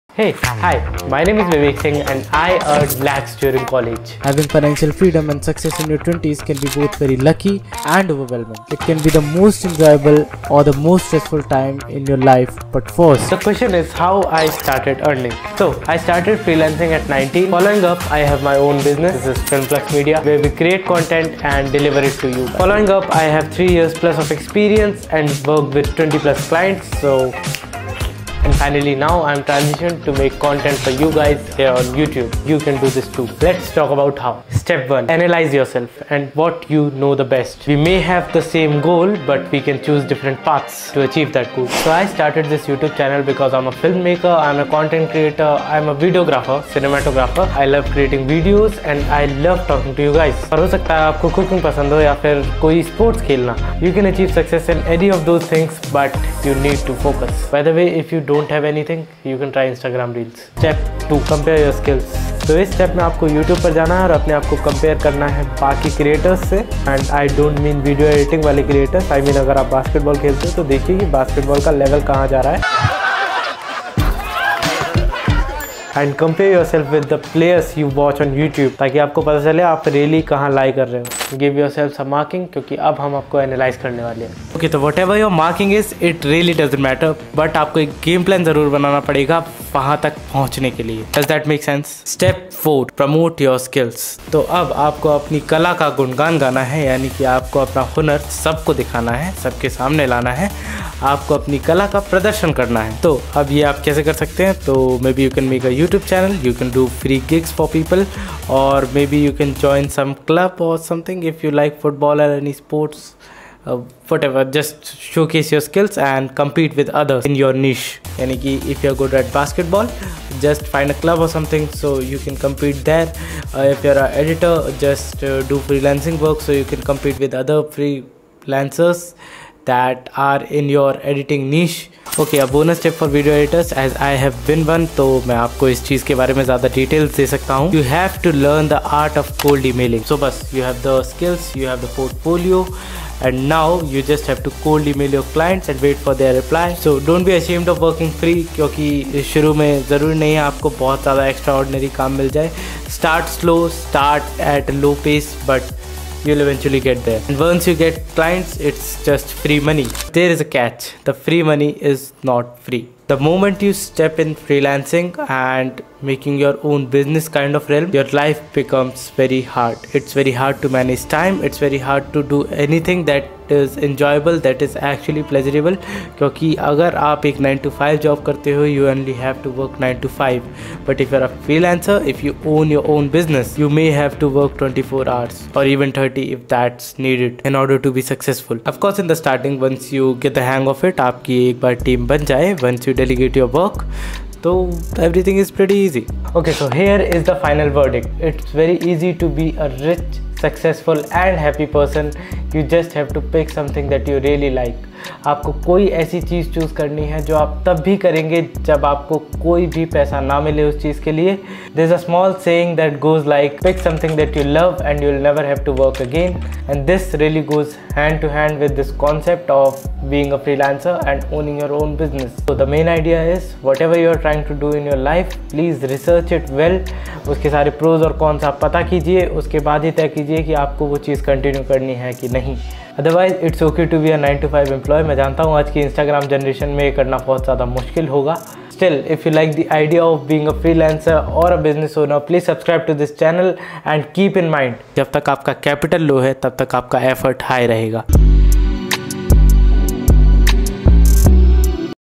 How I Earned lakhs during College
In this inspiring story,
a resourceful college student who discovered a unique and lucrative work-from-home opportunity that transformed their financial situation. Discover the secrets behind their success as they share valuable insights, practical tips, and the dedication it took to turn a simple idea into a lucrative venture. Whether you're a student seeking financial independence or anyone looking for new remote work opportunities, this narrative will provide you with invaluable lessons and inspiration to embark on your own journey to financial success. Don't miss out on this tale of determination, innovation, and triumph!